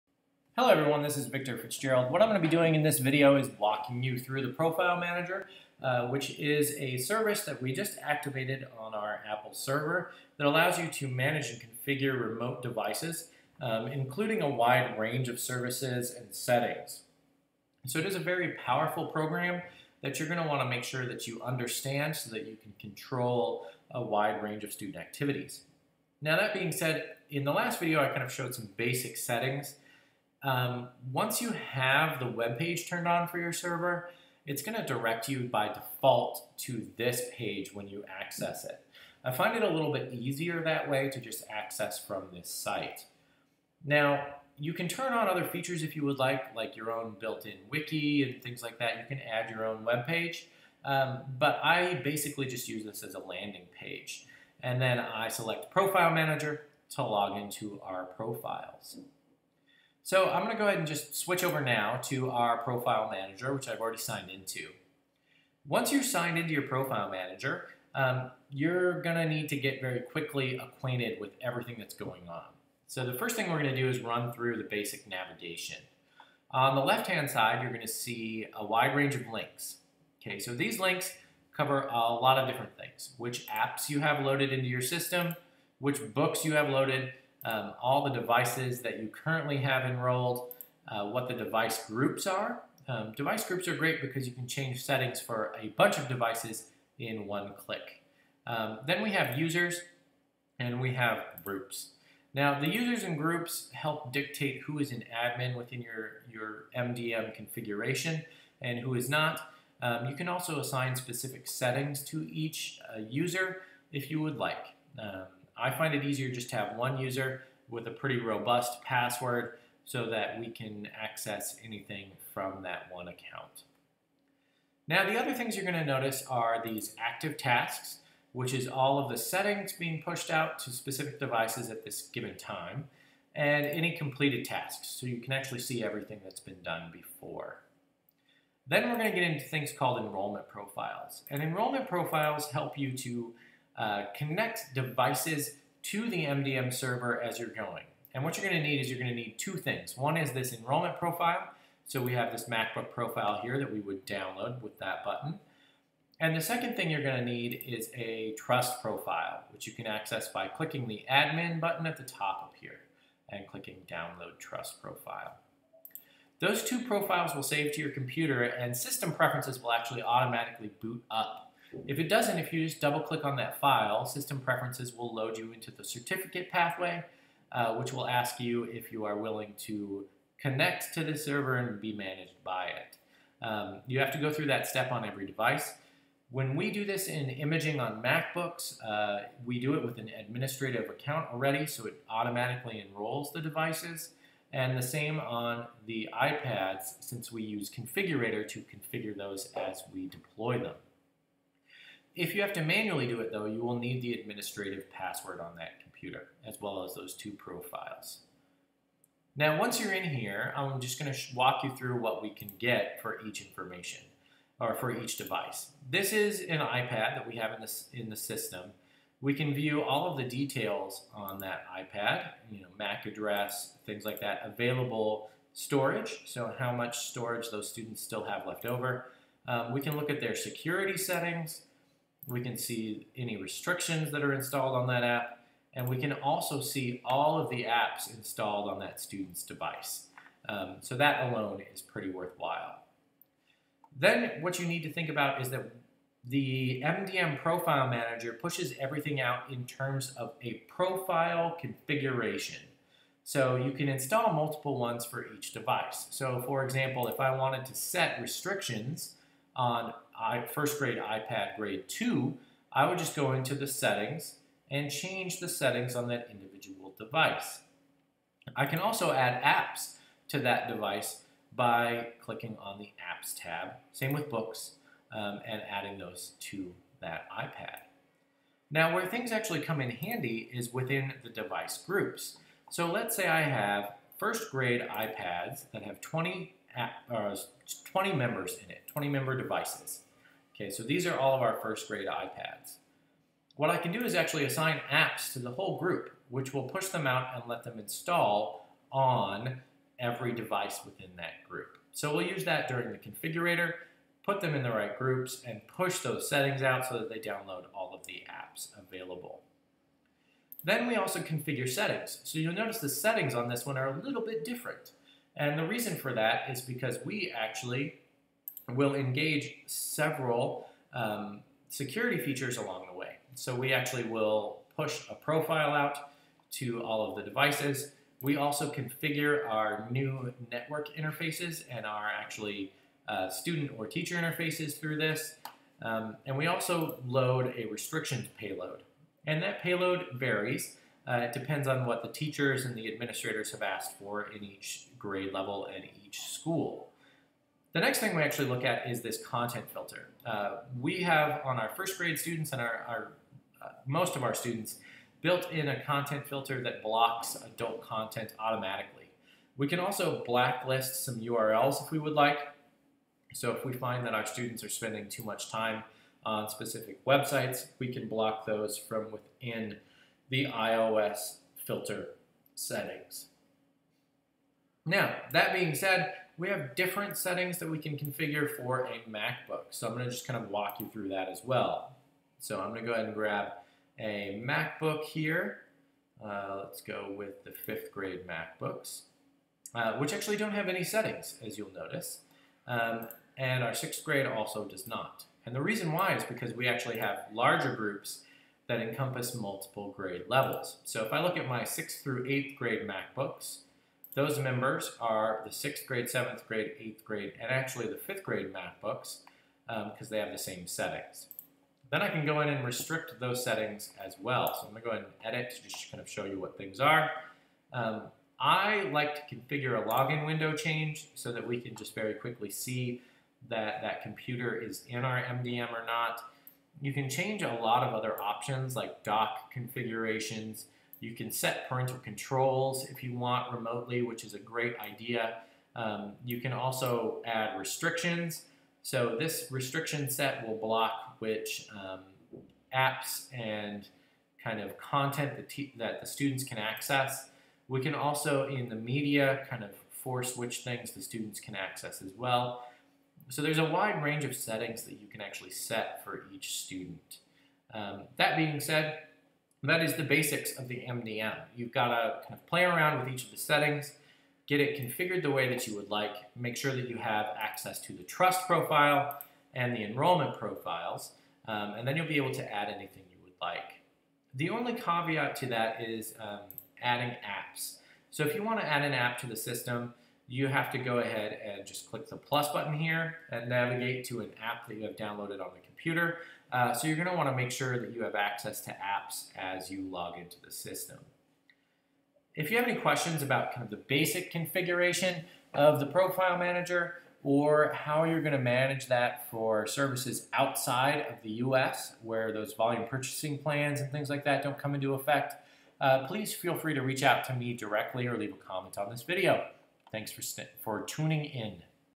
Introduction to Profile Manager
This is a brief tour of some of the settings and features within the Apple MDM Server Profile Manager.